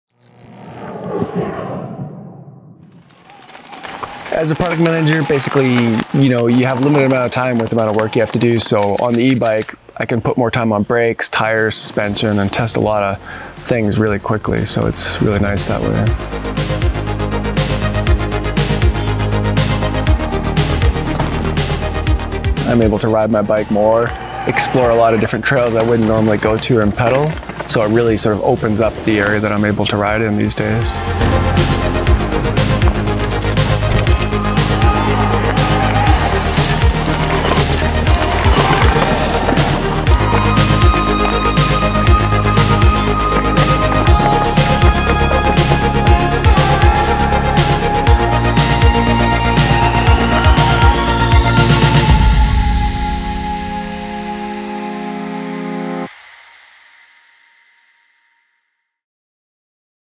Remote 160 Chapter 1 - Trevor Porter
Trevor Porter is our electric bike product manager. Based out of Squamish, BC, Trevor is constantly testing out different eBike setups, performance across varying terrain, and just how far he can go. Riding eBikes has proven to be an excellent and efficient way of learning which components are the best for various bikes-both electric and non.